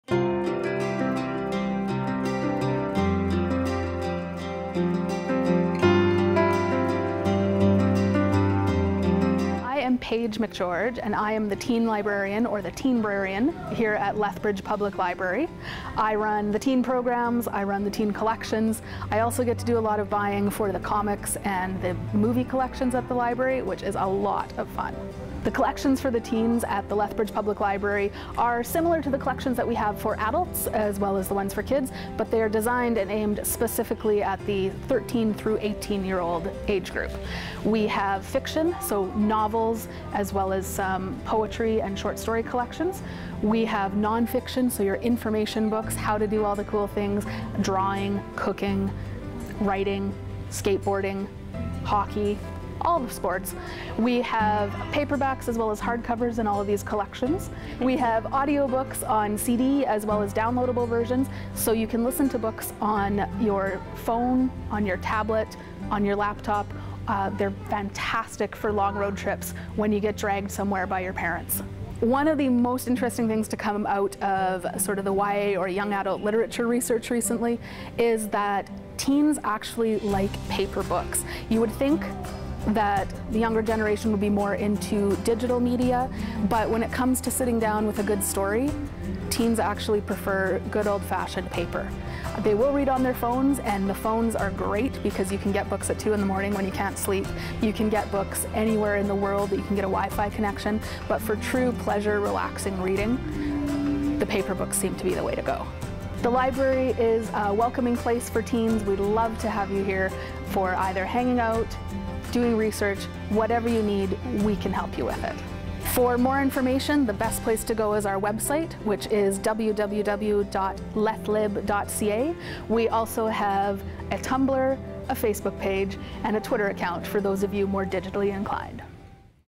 Connected To YOU - Lethbridge Public Library - Teen Collection
In this series, we explore the ways that a the local Library is still relevant in 2016